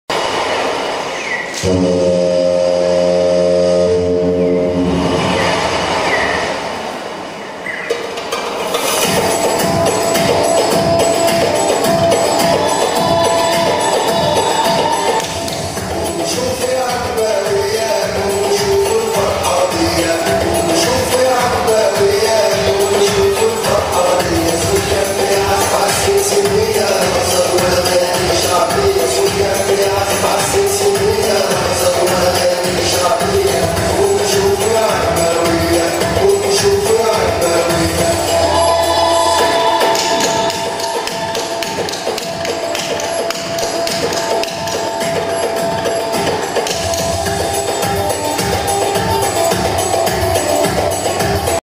Елена Дударская Египетский фольк Симсимия
Елена Дударская. «Египетский фольклор- Симсимия! История рождения стиля, характерные особенности, костюм, музыка, лексика. На примере постановки»